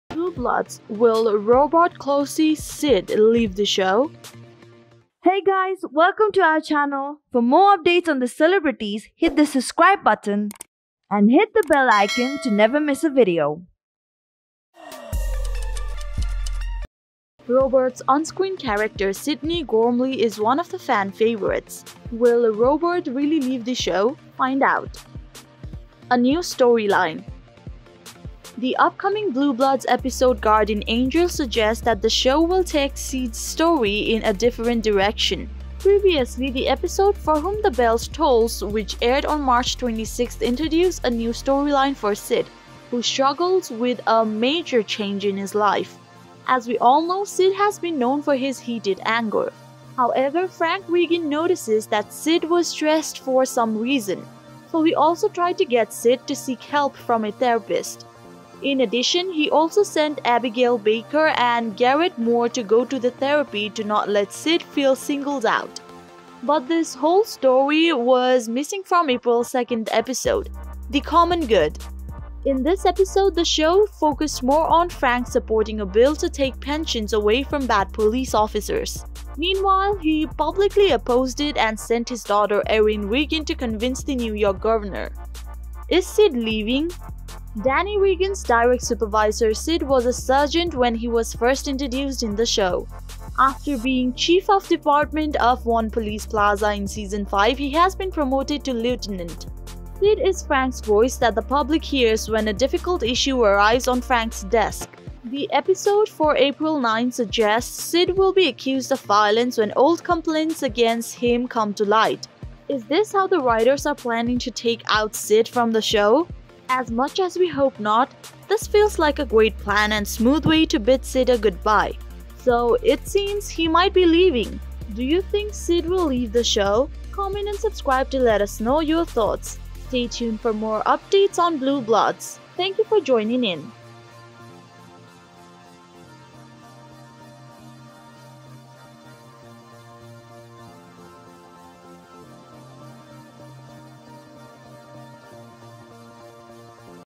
Blue Bloods Spoilers: Is Robert Clohessy “Sid” Leaving The Show?
Blue Bloods April 9 episode "Guardian Angels" suggests that Sidney "Sid" Gormley will be accused of excessive force as past complaints come to light. The fans are really curious to know if Robert Clohessy "Sid" is going to leave the show. As Sid has been introduced with a new story, he has been stressed lately. Meanwhile, Frank Reagan makes him seek help from a therapist even though he doesn't want to. So what will happen to Robert on-screen character, Sid? Find out more.